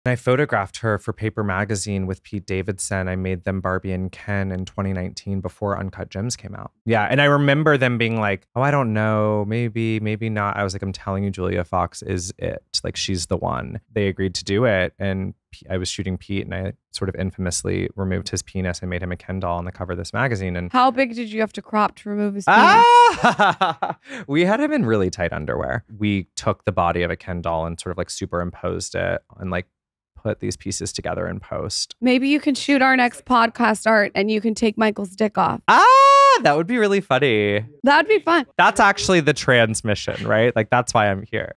How Julia Fox was Discovered - Tommy Dorfman
Today we're sitting down with actress, writer, photographer, and director, Tommy Dorfman. Tommy graduated with a degree in acting from Fordham University in 2015. First known for her role as Ryan Shaver on 13 Reasons Why, Tommy has since worked in television, film, and theater - recently starring in Jeremy O. Harris' hit play Daddy at The Signature. In this episode Tommy shares here experience as a trans person and we discuss all things trans rights and perspectives. We dive into how to have an impactful conversation surrounding the LGBTQ community, how to be an ally and ask questions, and what people can do to support the trans community. We also dive into her experience in childhood, how she was bullied from a young age and hid her identity for years. struggled with substance abuse & how she finally decided to own her identity.